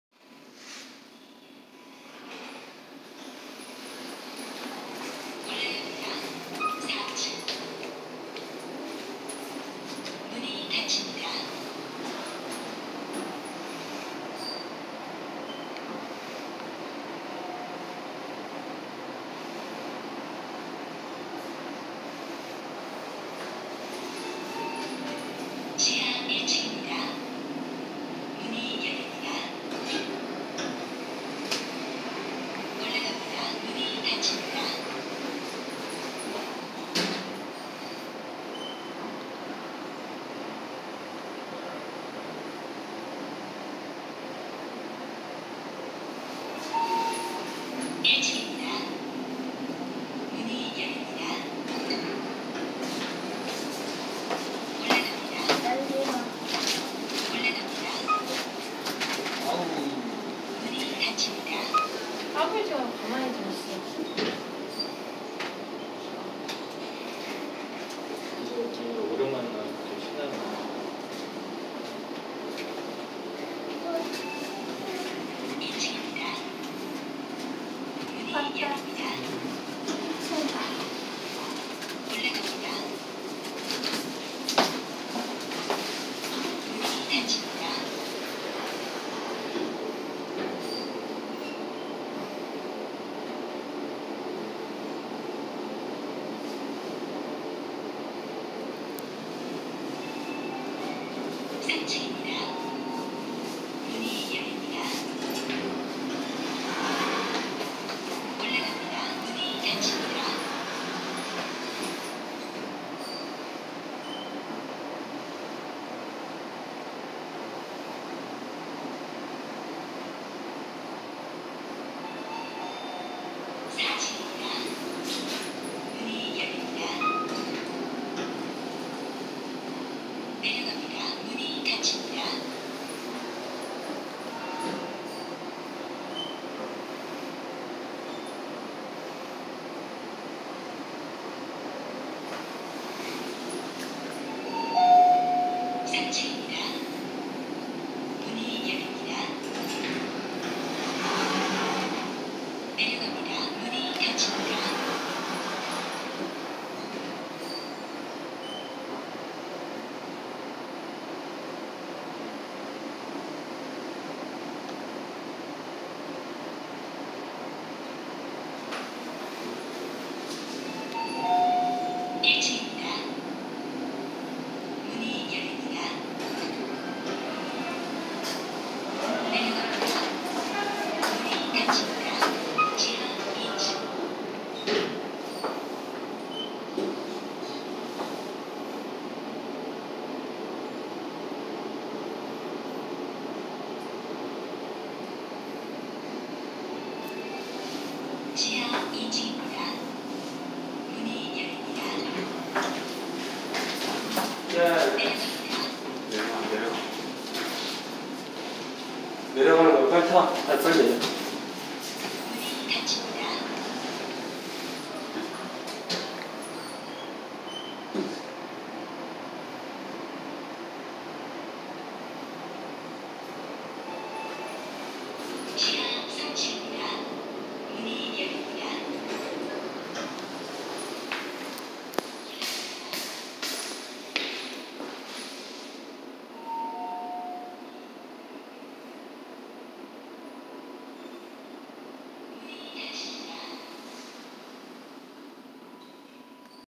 경기도 용인시 처인구 중부대로 1294 역북동 더와이스퀘어 1호기 현대엘리베이터 탑사기
1호기 지하 3층 ~ 4층

지하 4층은 공사 중이라서 폐쇄되었습니다 ㅠㅠ